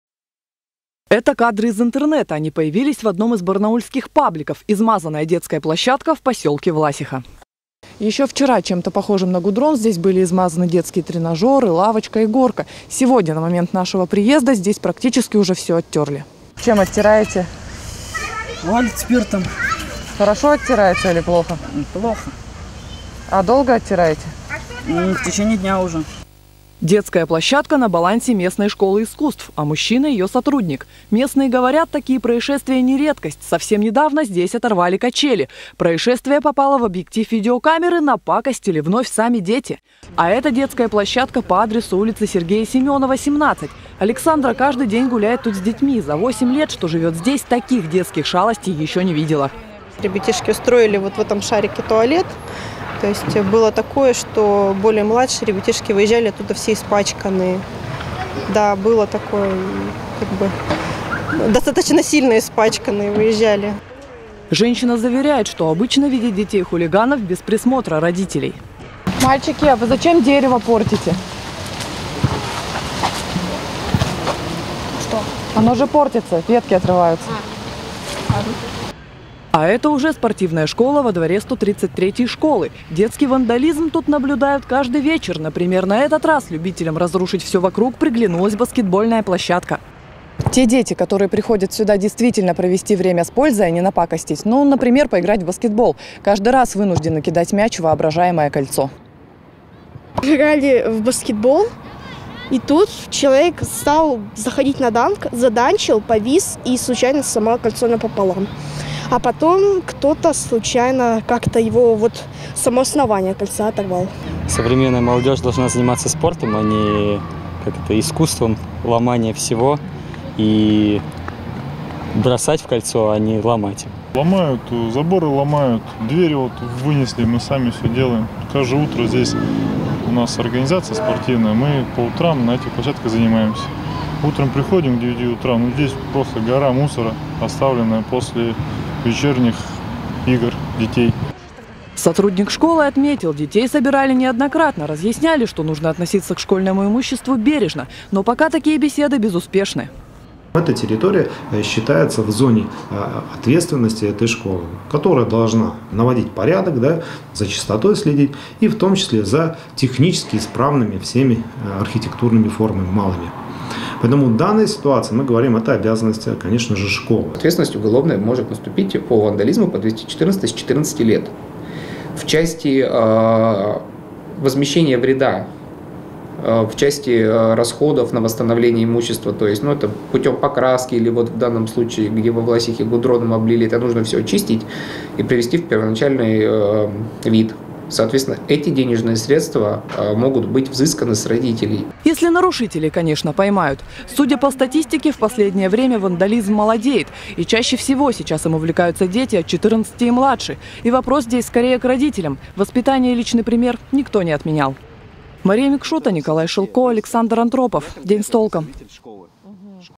Игровые площадки в барнаульских дворах атаковали дети-вандалы
В Барнауле участились случае вандализма. Наша съемочная группа прогулялась по детским площадкам и узнала, - как играют дети в недетские игры.